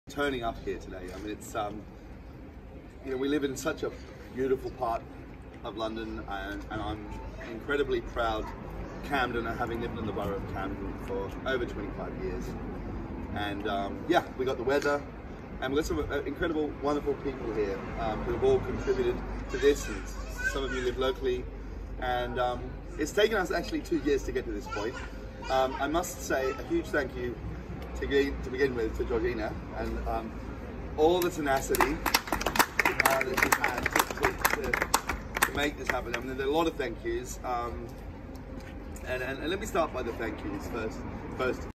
Founder of Camden Clean Air unveiling a new parklet in Belsize, Camden
The owner of England's Lane and founder of CamdenCleanAir, Jeffrey Young welcomes guests at the unveiling of the first parklet in Belsize with @camdenist, @camdengiving412, camdencouncil, @LimeBikes and Scooters and more.